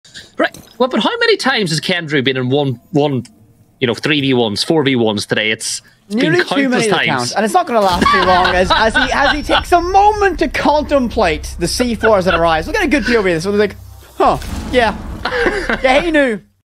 Outriders ShepparD sending a present to Kendrew
Nice play by Outriders ShepparD versus Secret Kendrew.